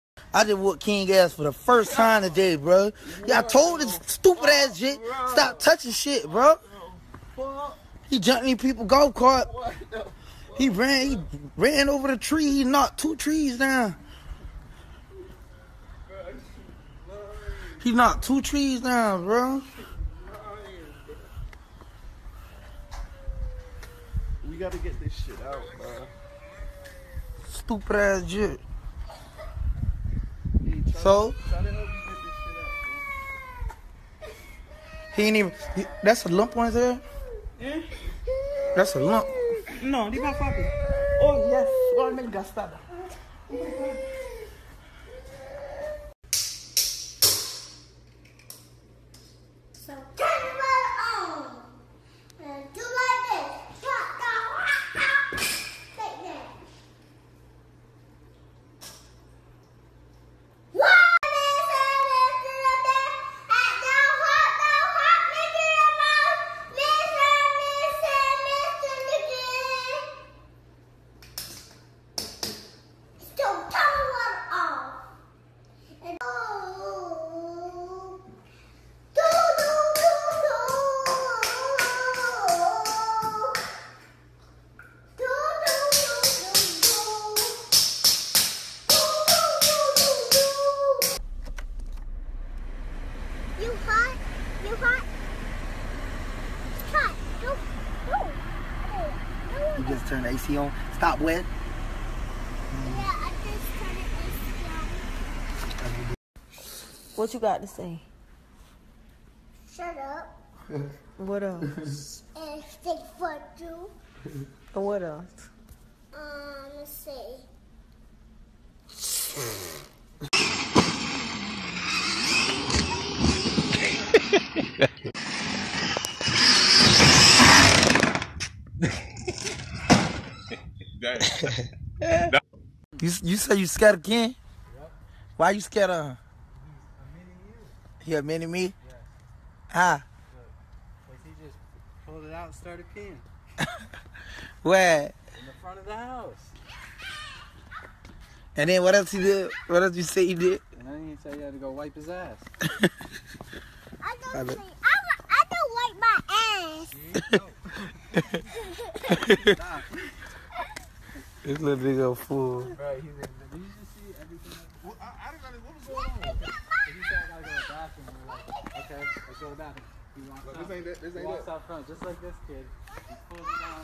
Kodak Black GOES OFF on His Son For Crashing Golf Cart into The TREES!
KodakBlack GOES OFF on His Son For Crashing his Golf Cart into The TREES!